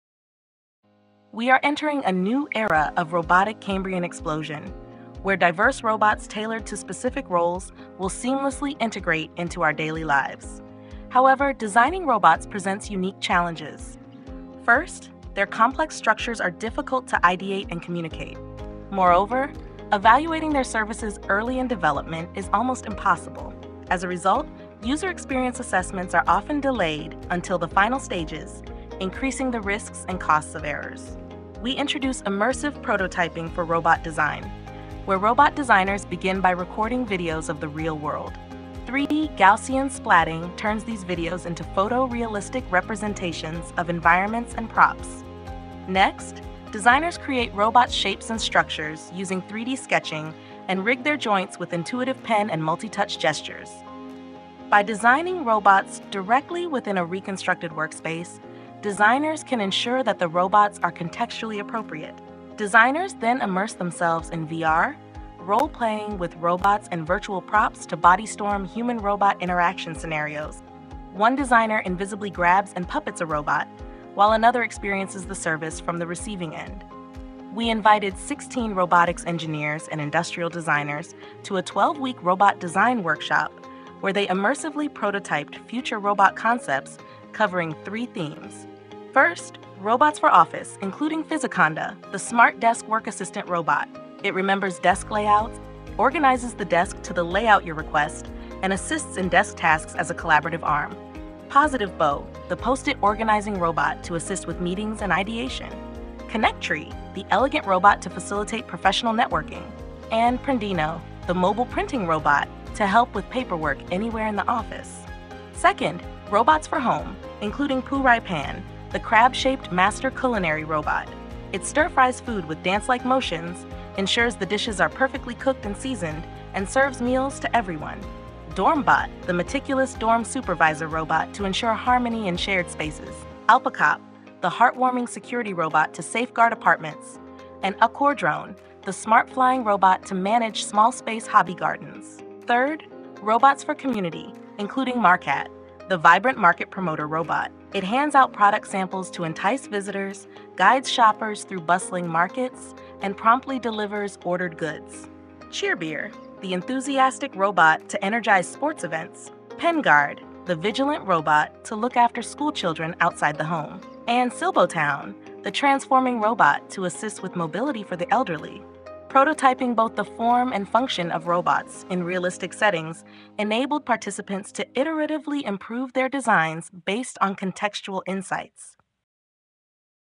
Immersive prototyping for robot design (CHI 2025 LBW) - 3 min. presentation
CHI 2025 Late-Breaking Work
Immersive prototyping for robot design with 3D sketching and VR acting in reconstructed workspace

Rapid advances in robotics and AI technologies are opening the possibility of a wide range of commercial robot products with diverse shapes, sizes, and structures specialized for various environments, contexts, and roles. This trend calls for innovative tools and methods for designing robots as products. In this study, we propose a novel immersive prototyping system and workflow for quickly and easily creating desired robot shapes and structures through 3D sketching in target environments, realistically experiencing their movements and services through VR acting in the same environments, and iteratively improving their designs based on contextual insights. We conducted an extended robot design workshop with participants from backgrounds in robotics engineering and industrial design. The results show that the proposed system and workflow can help robot designers produce highly creative and compelling design outcomes, while also identifying areas for future improvement.